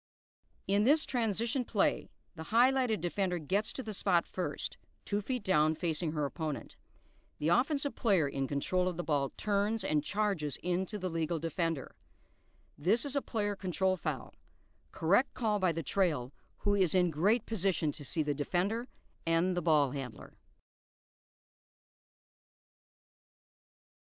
F 108 Block Charge Charge in Transition
The defender establishes legal guarding position